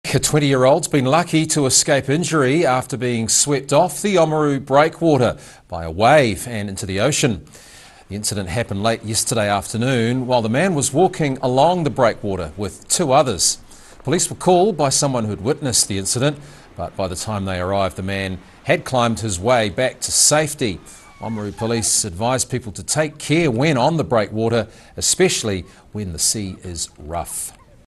Man swept off Oamaru breakwater
A 20-year-old has been lucky to escape injury after being swept off the Oamaru breakwater by a wave and into the ocean.